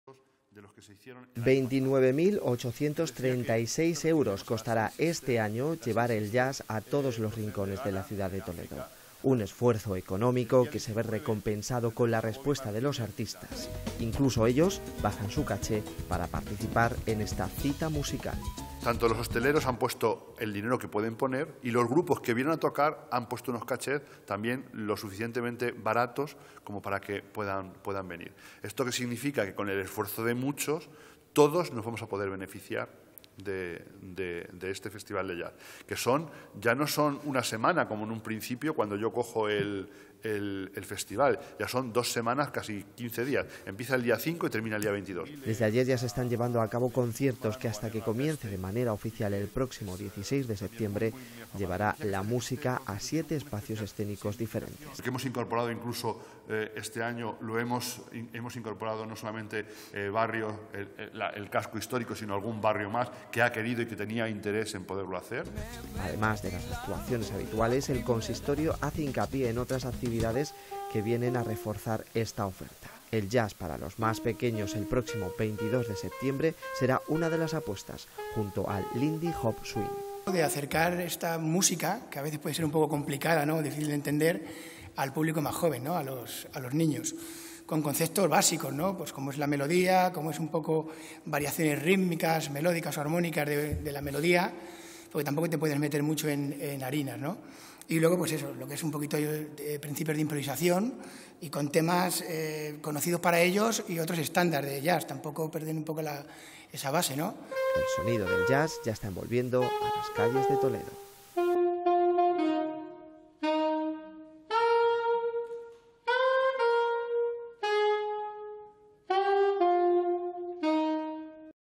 El ayuntamiento de Toledo apuesta por una nueva edición del Festival de Jazz
El ayuntamiento de Toledo apuesta por una nueva edición del Festival de Jazz. Con menor presupuesto, se mantendrán los conciertos, potenciando la participación de músicos toledanos.
29.836 euros costara este año, llevar el jazz a todos los rincones de la ciudad de Toledo. Un esfuerzo económico, que se ve recompensado con la respuesta de los artistas. Incluso ellos bajan su cache para participar en esta cita musical.
Desde ayer ya se están llevando a cabo conciertos, que hasta que comience de manera oficial el próximo 16 de septiembre, llevara la música a 7 espacios escénicos diferentes.
Además de las actuaciones habituales, el consistorio hace hincapié en otras actividades que vienen a reforzar esta oferta. Jazz para los más pequeños el próximo 22 de septiembre, será una de las apuestas, junto al LINDY HOP SWING
El sonido del Jazz ya esta envolviendo a las calles de Toledo.